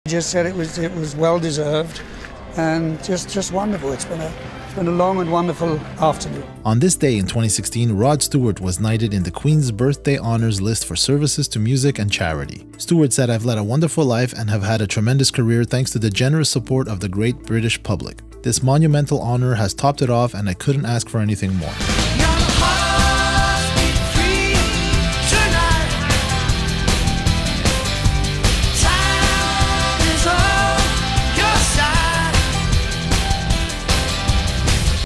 On This Day June 10 Rod Stewart was knighted in the Queen's Birthday Honours list
10 Jun 2016
Rod Stewart was knighted in the Queen's Birthday Honours list for services to music and charity. Stewart said: "I've led a wonderful life and have had a tremendous career thanks to the generous support of the great British public. This monumental honour has topped it off and I couldn't ask for anything more."